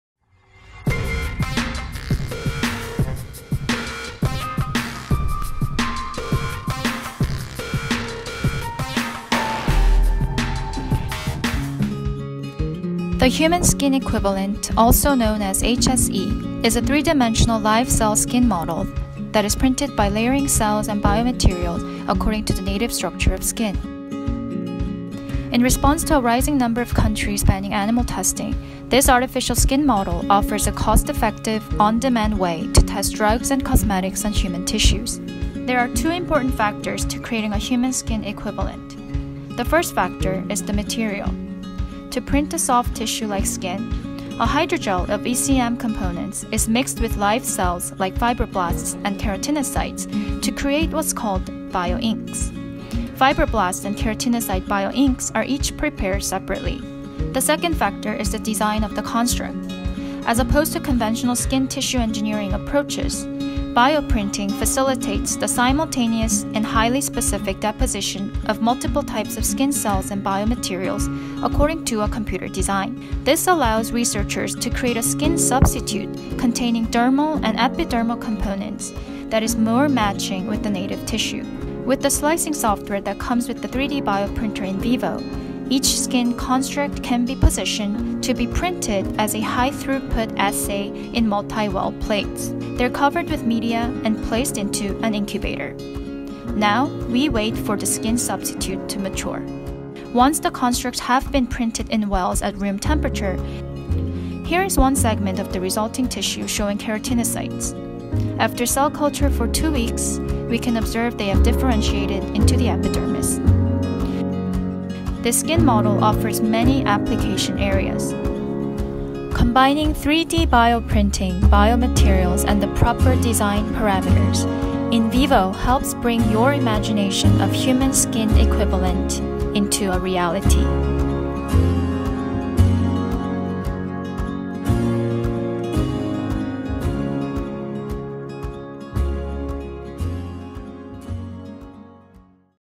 A Solution to Replace Animal Testing
an in vitro skin model composed of multi-layered human cells, commercially applicable as a clinical skin substitute or a model for screening of drugs and cosmetics.